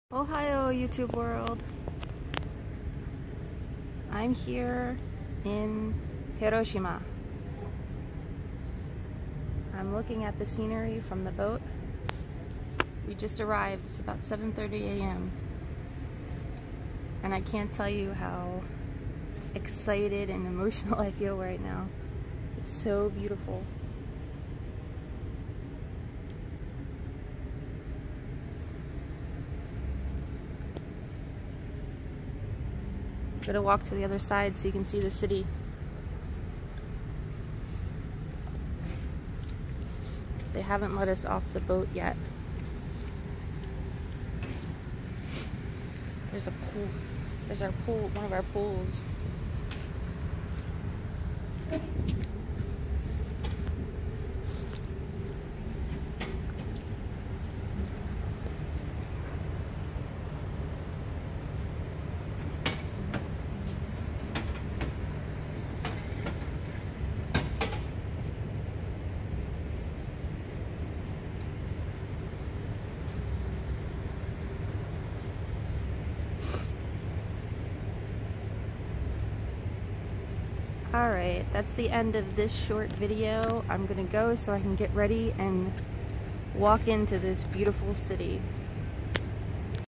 Hiroshima July 13, 2018.
This is a view from our ship in Hiroshima. It's land scape was breathtaking.